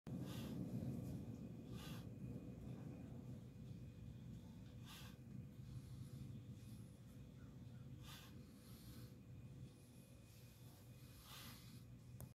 B9 brain lights rainbow pattern
I've been playing with the FastLED library, trying different movement patterns, speeds, and colors; this is my favorite so far...